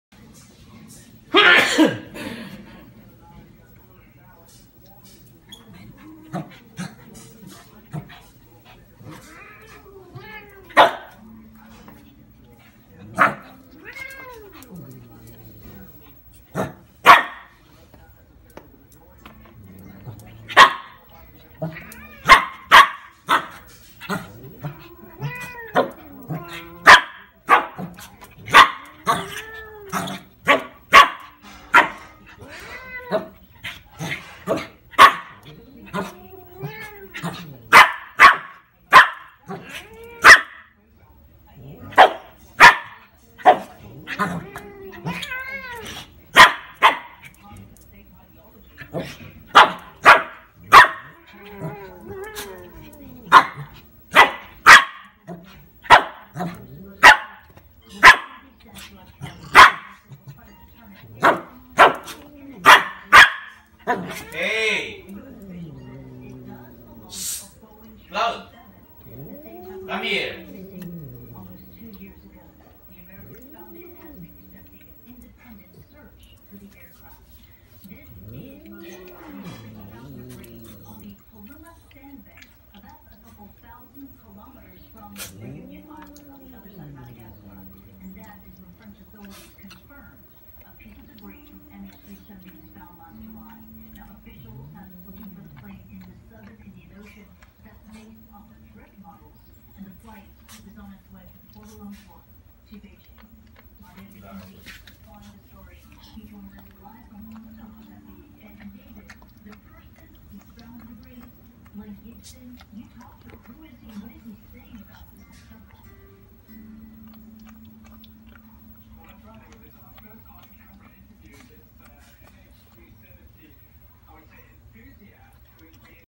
Cloud vs Khatie Round 2
1 year old Cloud the Shih-Tzu surrenders to 9 year old Khatie the Persian